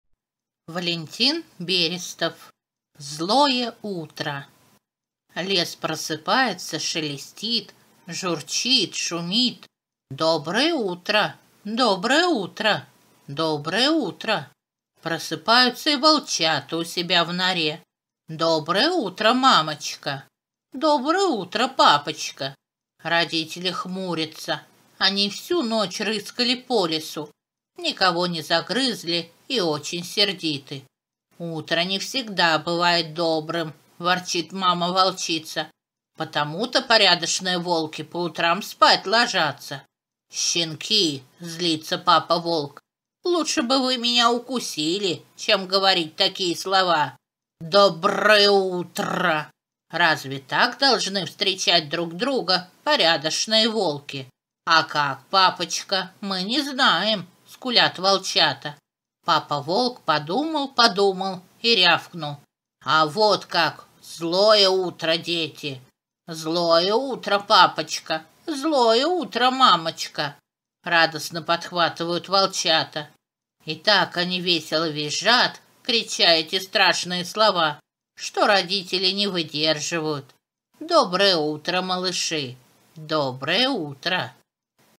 Аудиокнига Злое утро Валентин Берестов
Злое утро — короткая сказка Валентина Берестова, которую стоит прочесть вместе с детьми всех возрастов. В ней показана картина просыпающегося леса, где каждый кустик, деревце и травинка шепчут своим соседям приветственную фразу: «Доброе утро!». Однако есть в лесу семья, где никто не желает друг другу доброго начала дня. Что это за семейство, прочтите вместе с детьми из небольшого произведения. Оно учит вежливости и показывает неизмеримую глубину родительской любви.

Карта сбербанка: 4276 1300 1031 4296